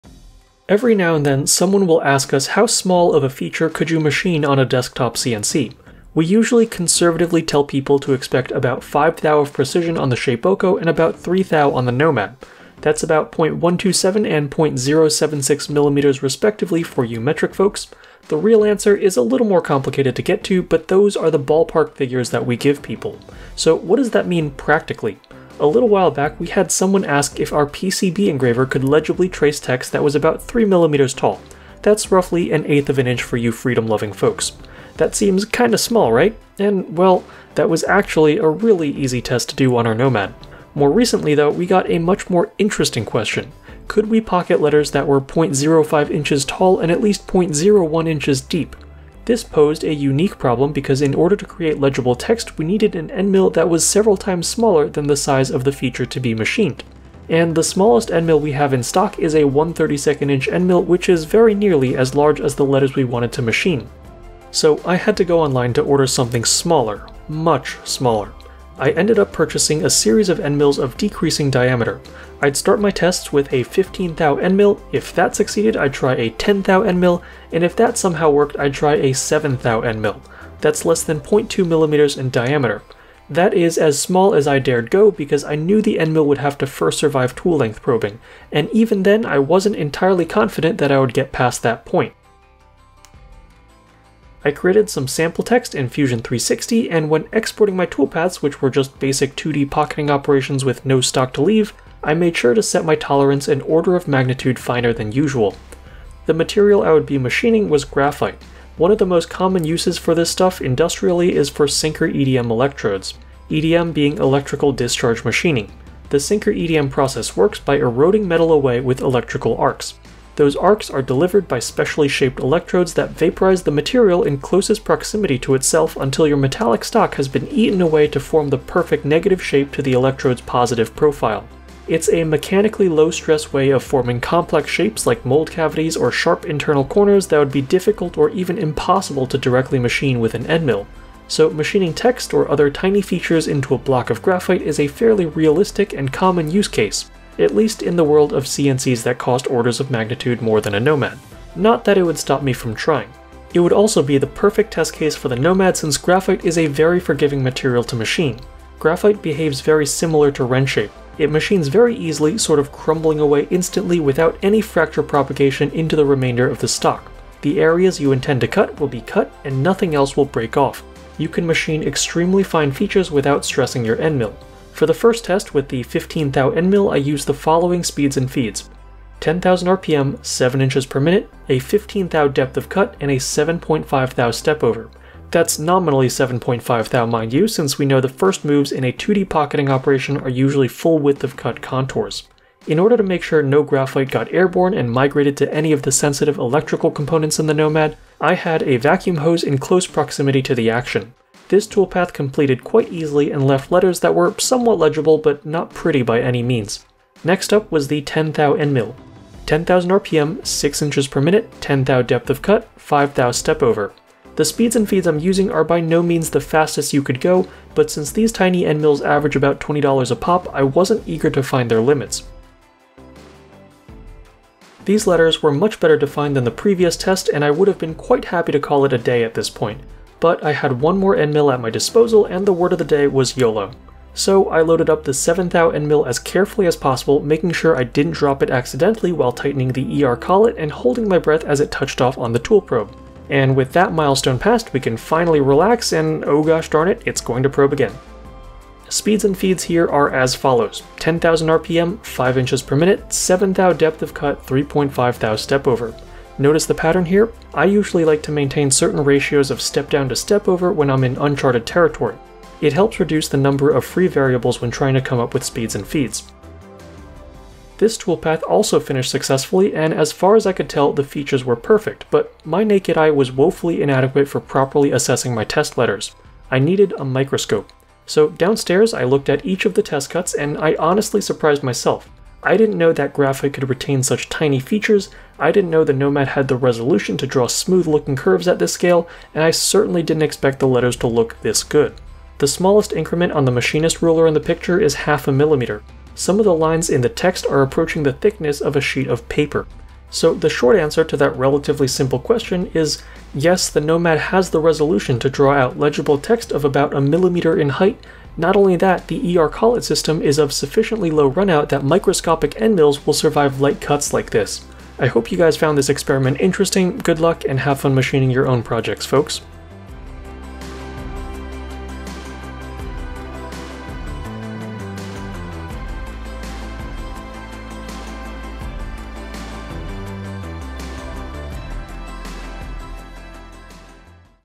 What's the Smallest Thing You Can Machine on a Nomad CNC?
We get asked all the time if you could machine or engrave text at some given resolution.  Well, with the right endmills, you can get pretty darned small. Here are some 0.05" tall letters we machined into graphite.

Learn more about our lines of Desktop CNC machines at carbide3d.com